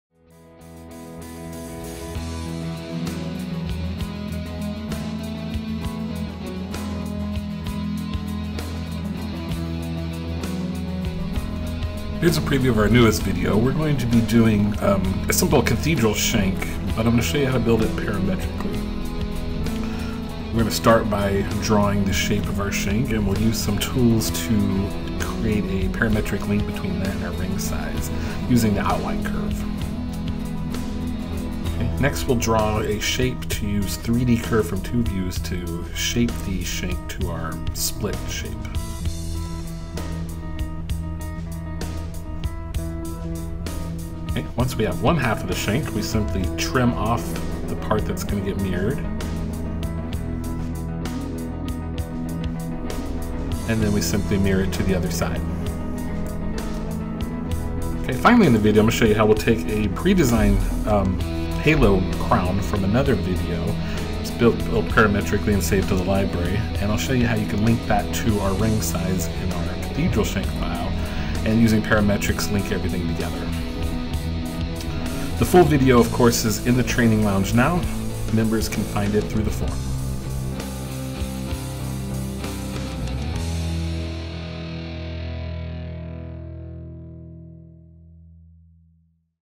Cathedral Shank Preview
Learn to design a parametric cathedral shank with 3DESIGN and assemble it with a halo crown pre-designed in an earlier 3DESIGN video. As always, the full video can be found in the 3DESIGN training lounge. for more information on how to revolutionize your Jewelry business.